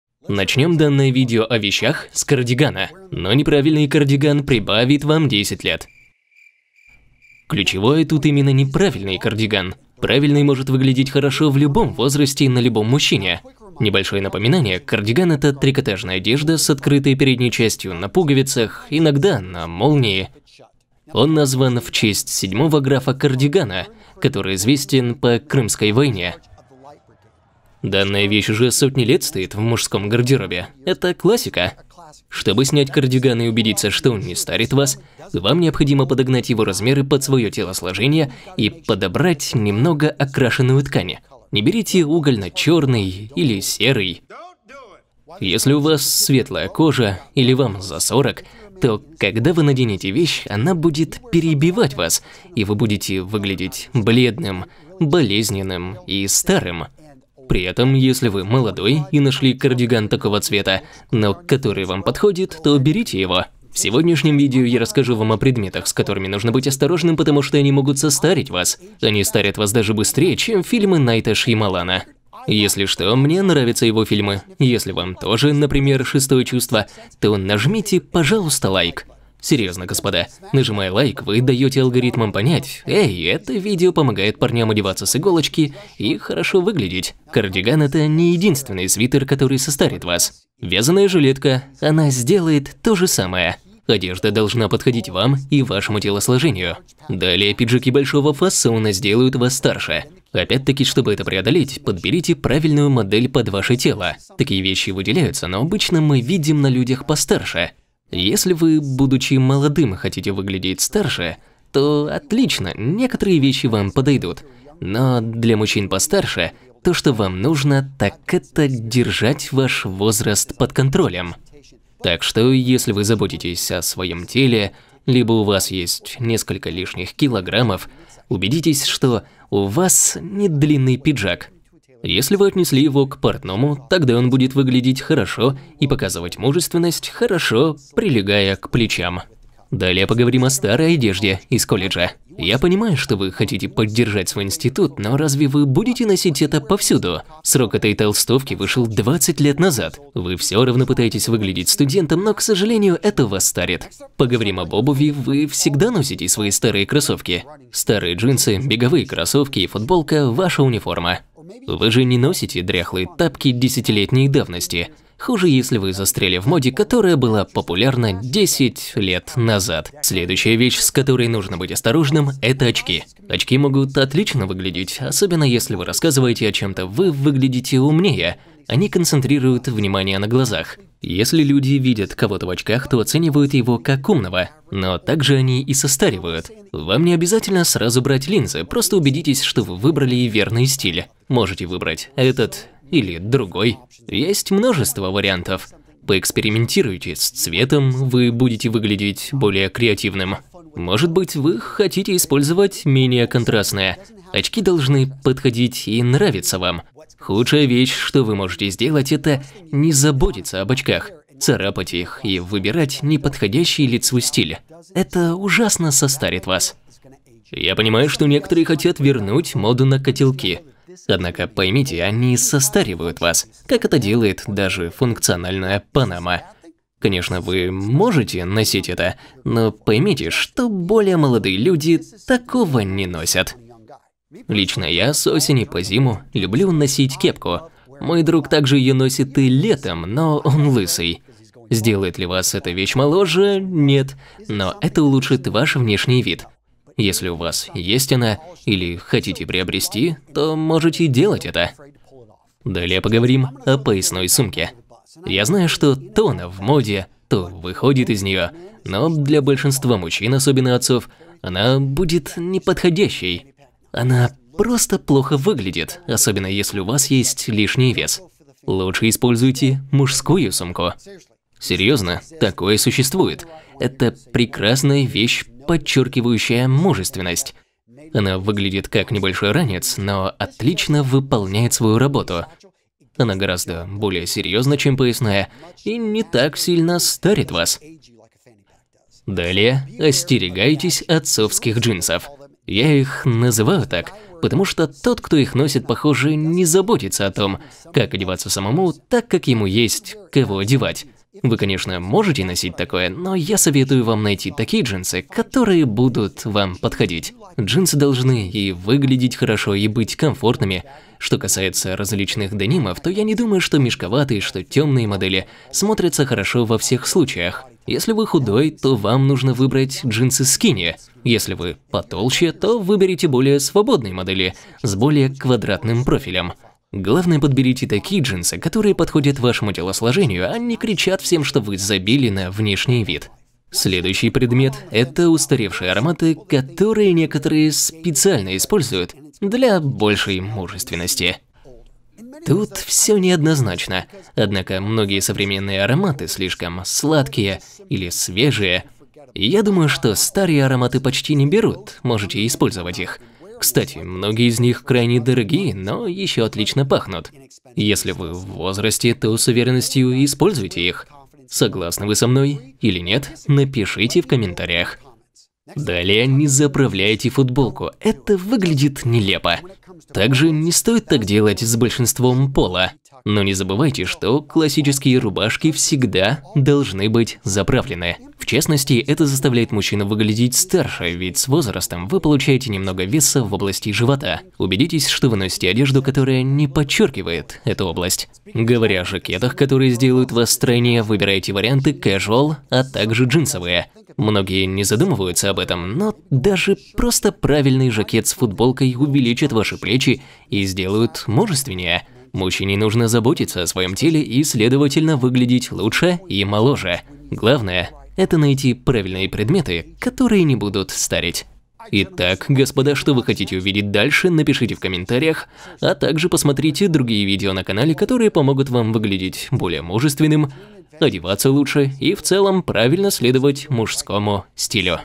Прекратите носить эти 7 ВЕЩЕЙ (из-за них вы выглядите старым)
В этом видео Антонио расскажет о 7 вещах, которые делают вас старым.

Таймкоды:
00:00 Вещи, которые делают вас старым
00:01 Кардиган
1:29 Вязаная жилетка
1:39 Пиджаки большого фасон
2:23 Старая одежда из колледжа
2:35 Устаревшая обувь
2:50 Очки
3:39 Старомодные головные уборы
4:15 Поясная сумка
4:53 Отцовские джинсы
5:37 Устаревшие ароматы
6:07 Заправлять футболку